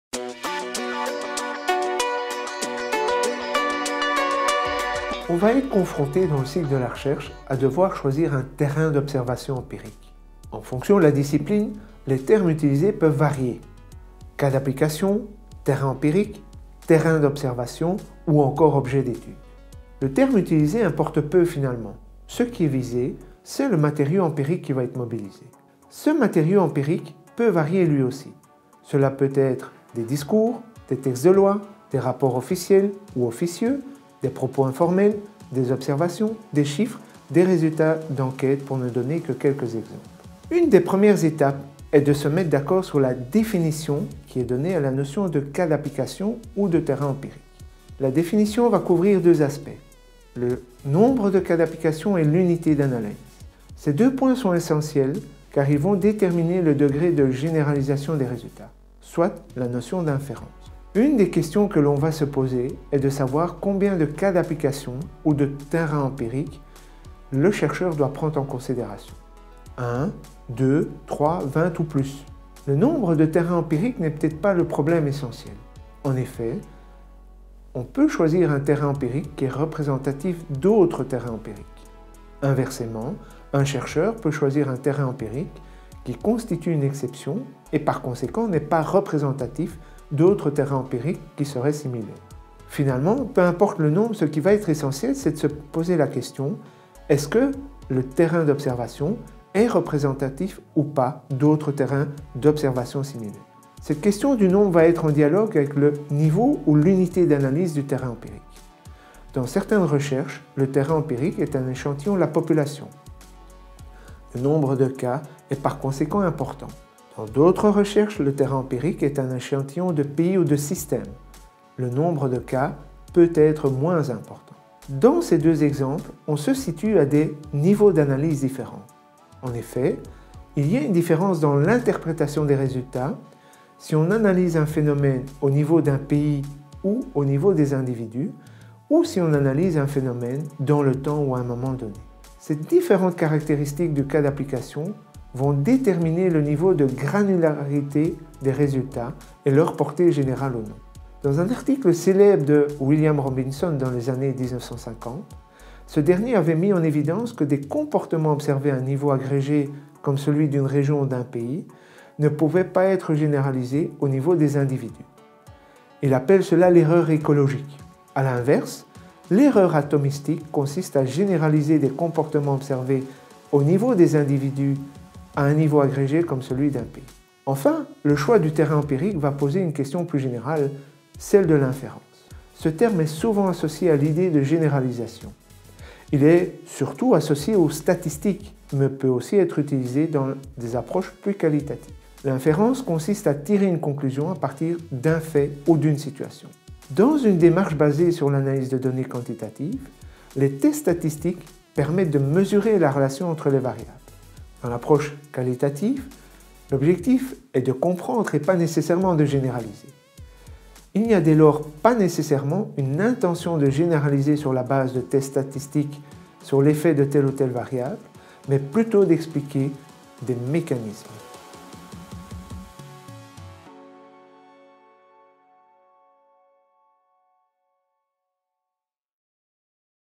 19.Quel cas d'application mobiliser dans sa recherche ? | Pierre Baudewyns | Louv23x
Cette vidéo est extraite du cours en ligne ouvert à tous (MOOC) 'Pratiquer la méthode de recherche en sciences humaines et sociales' (Louv23x) produit par l'UCLouvain et disponible sur edX. Inscrivez-vous gratuitement